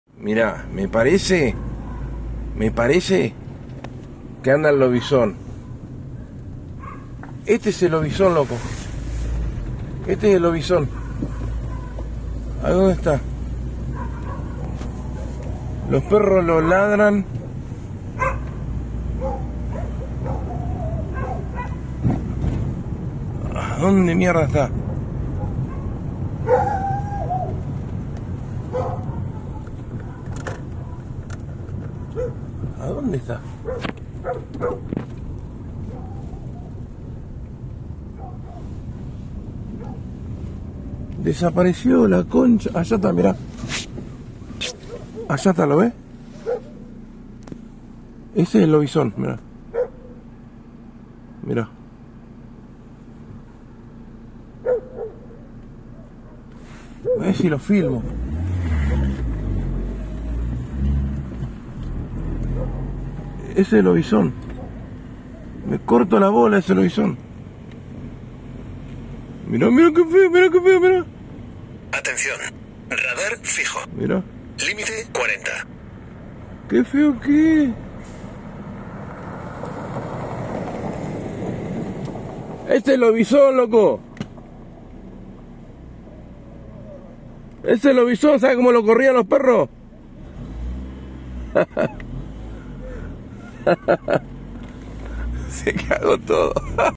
lobizon en José c paz
este lo filme yo cuando andaba haciendo Uber en José c paz a las 03 de la madrugada